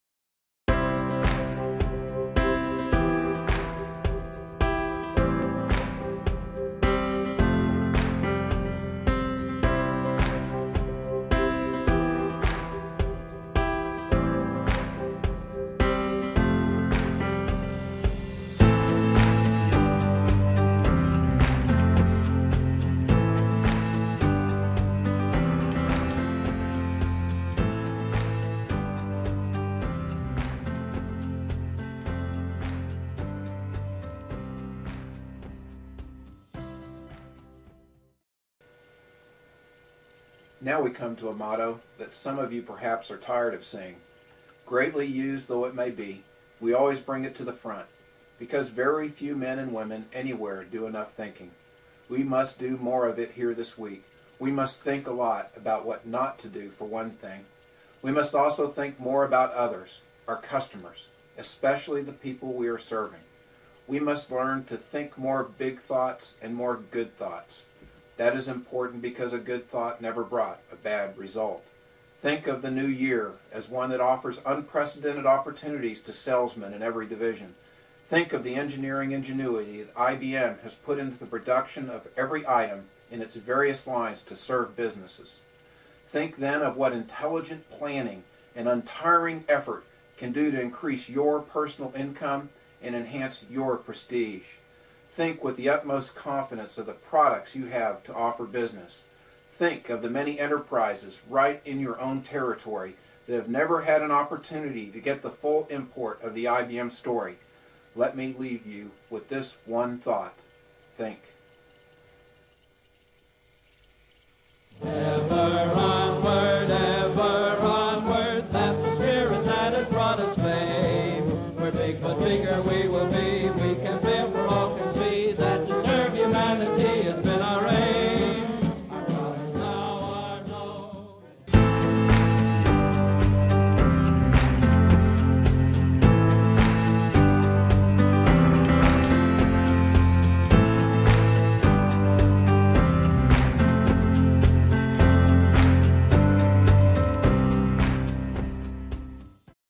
Thomas J. Watson Sr.: THINK
Last of the Tom Watson Sr. video series and, of course, it has to end with THINK. Here are a few choice quotes of Tom Watson Sr. on thinking:

- If a man thinks about his work—if he puts real thought into everything he does—he should be and will be forgiven for the mistakes he makes.

- If he earns the reputation of thinking carefully about everything he does, the mistakes a man does make will be excused.

- Thinking is what really makes an organization.

- But if a man tells me that he did not think, I do not like it. I do not like to overlook mistakes of that kind, because a man should think.

THINK!

Pete